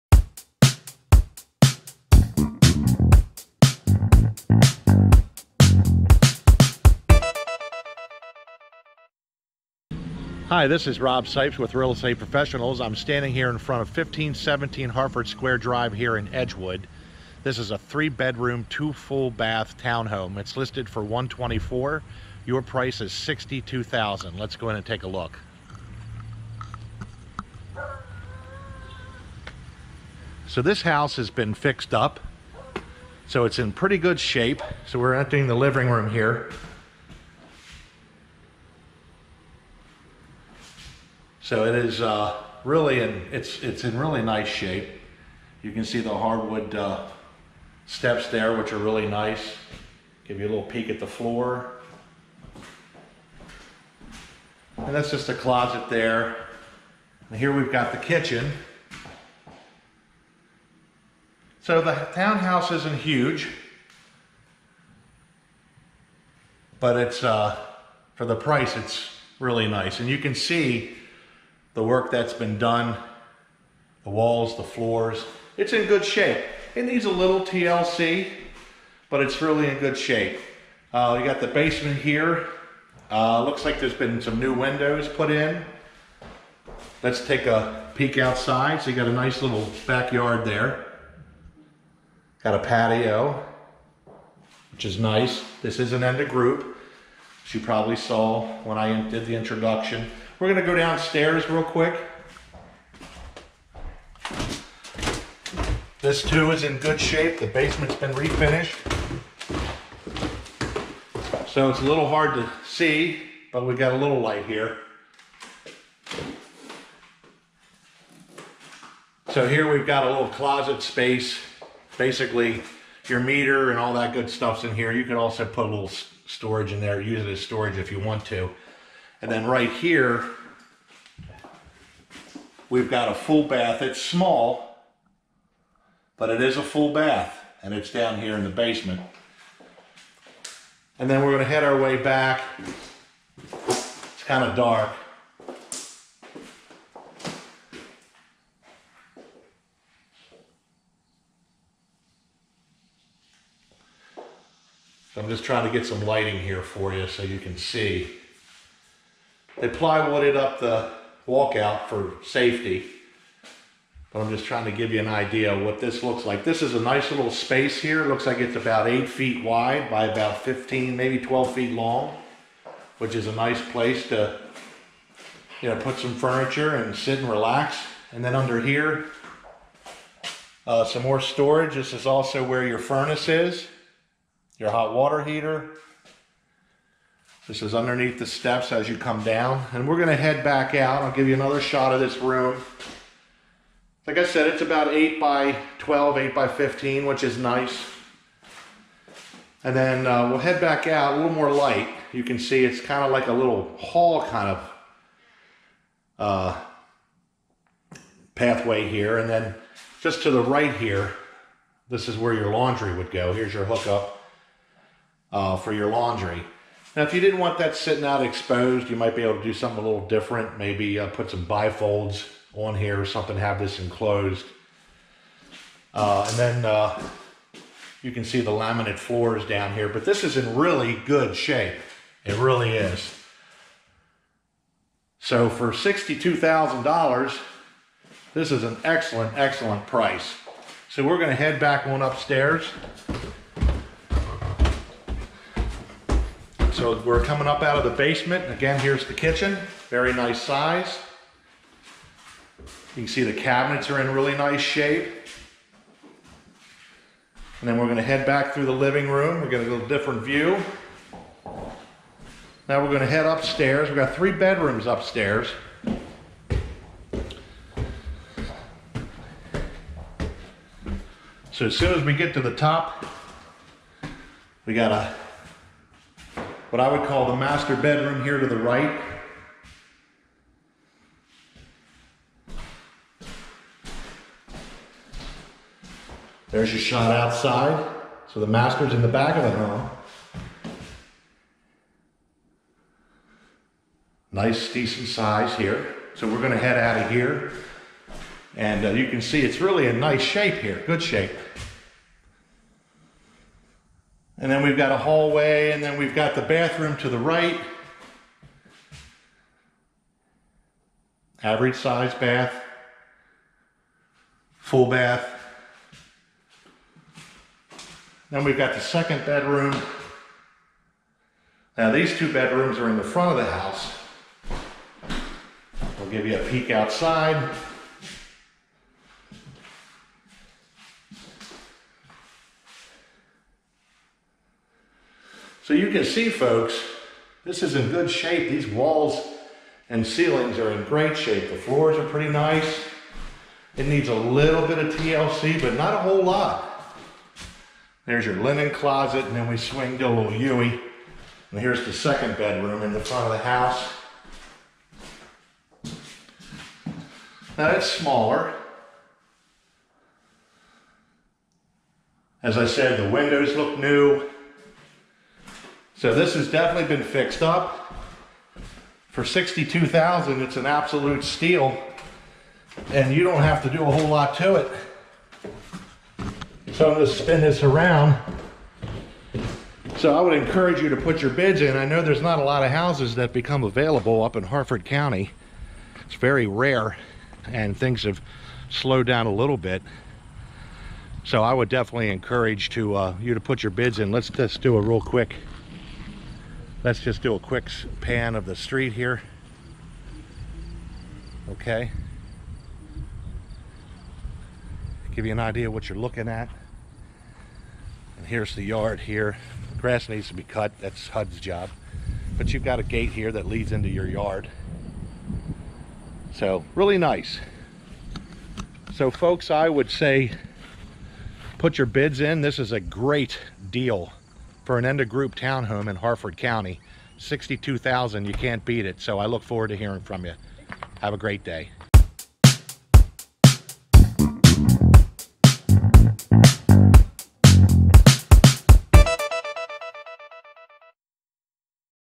50% Discount Home | $62,000 | 1517 Harford Square Dr | Edgewood, MD
September 4, 2020 – Good Neighbor Next Door home available at 1517 Harford Square Drive Edgewood, MD 21040

Appraised Value: $124,000
After 50% discount: $62,000

Law enforcement officers, teachers and firefighters only. Good Neighbor Next Door Program information available

Real Estate Professionals, Inc.
518 Eastern Blvd
Essex, MD 21221

HUD owned. Equal Housing Opportunity